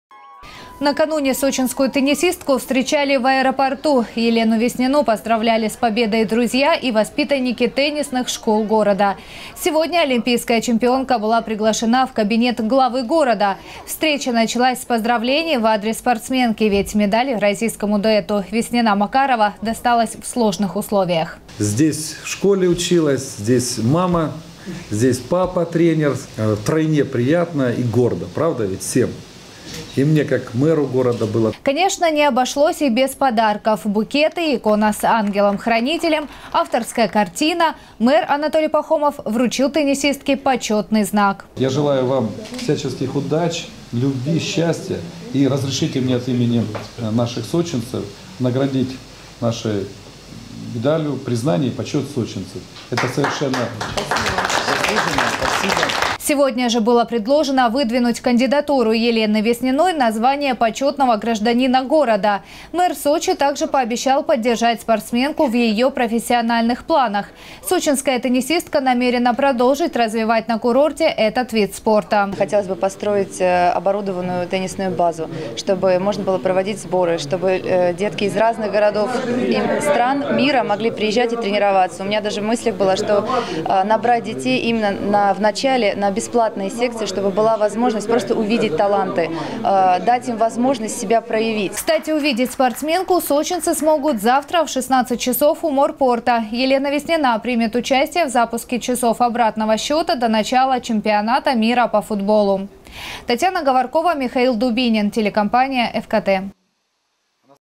Елена Веснина может стать почётным гражданином Сочи. Новости Сочи Эфкате
Об этом стало известно во время встречи мэра курорта Анатолия Пахомова с олимпийской чемпионкой в парном женском разряде по теннису на Бразильских летних Играх.
Сочи. Новости.
Телеканал Эфкате.